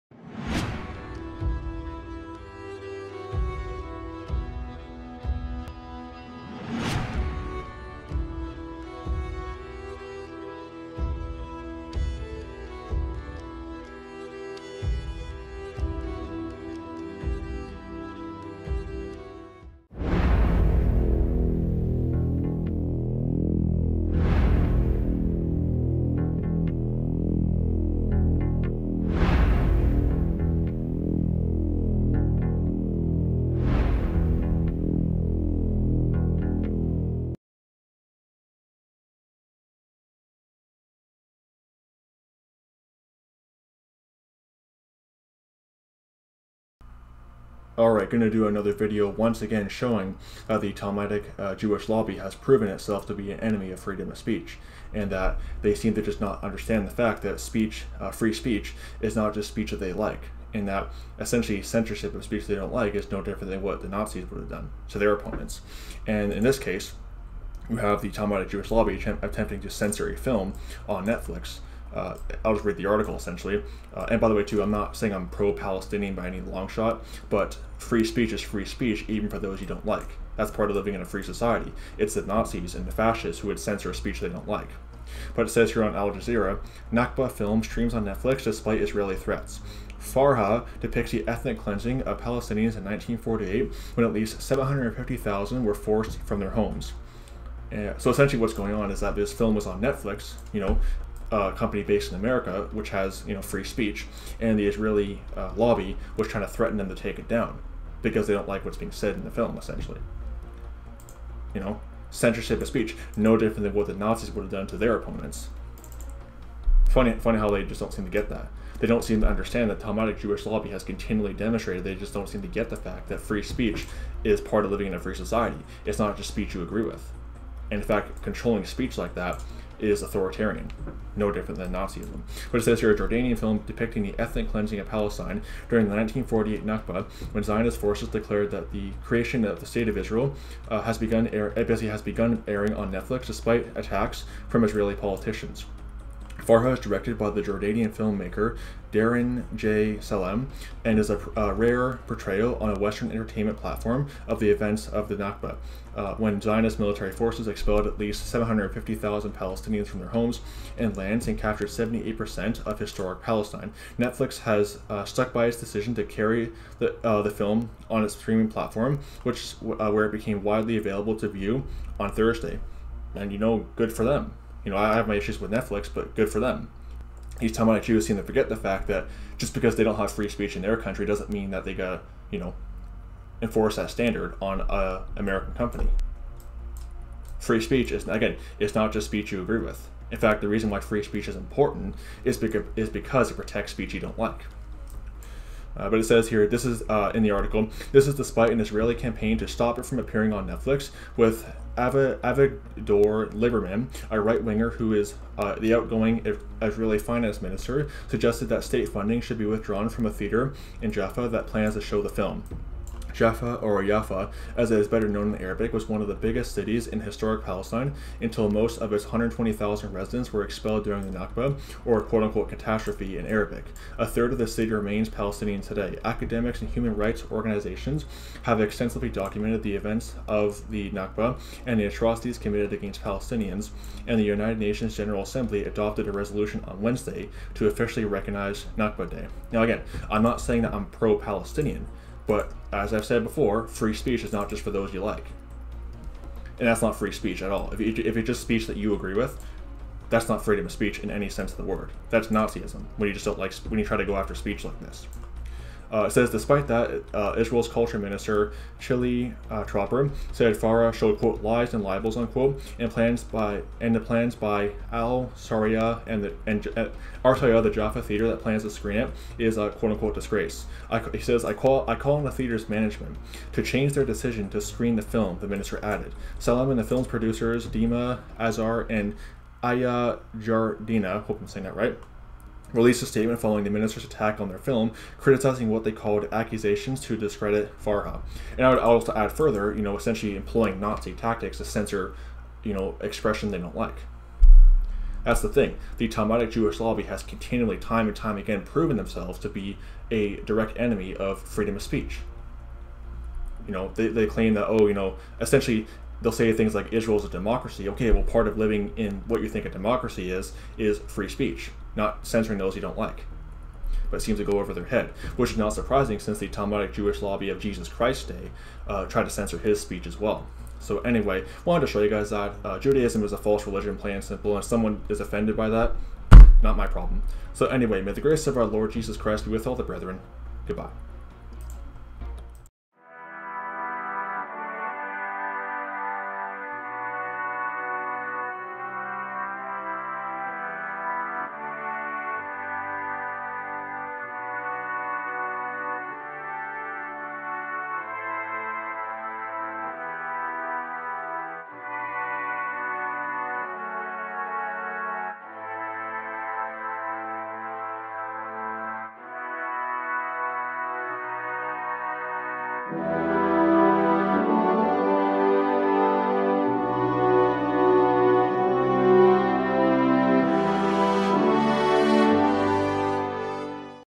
Talmudic Jewish Lobby Proven To Be Enemies Of Free Speech By Targeting Netflix Film
Talmudic Jewish lobby continually proves itself to be an enemy of free speech (they do so by employing Nazi tactics of trying to censor free speech of their critics and trying to censor speech they don’t like).